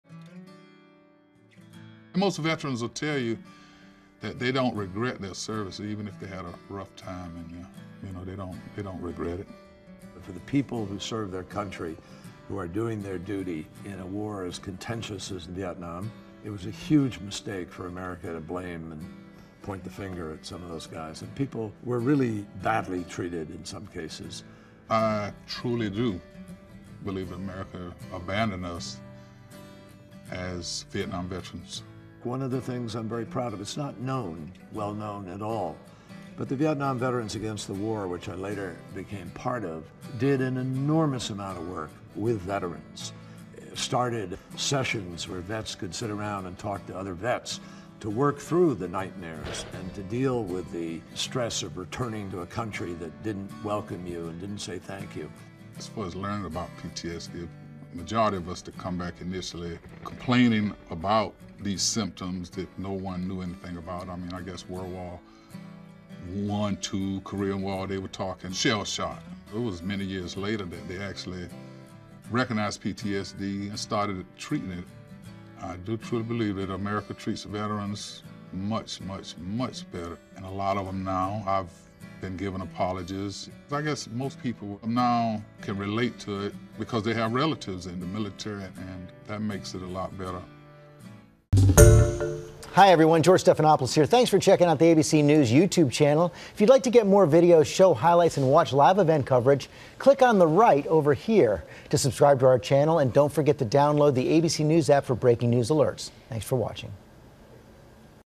Vietnam vets John Kerry, Tiney Corbett Jr on how the war changed their views
Though their stories differ, Kerry and Corbett learned to come to terms with his experience in his own way. They discuss what they went through then and how veterans are treated today.